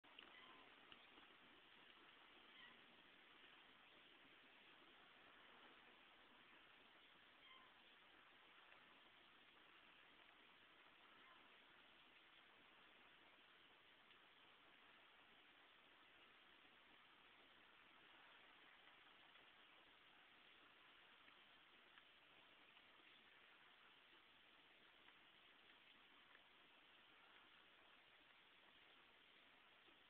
Felines - Part 4 of 5
These kittens used to be in a pot filled with soil and leaves, but I moved them to the laundry basket lined with a clean towel and old pair of blue jeans.  They seem to like it in the basket as opposed to the soil-filled pot.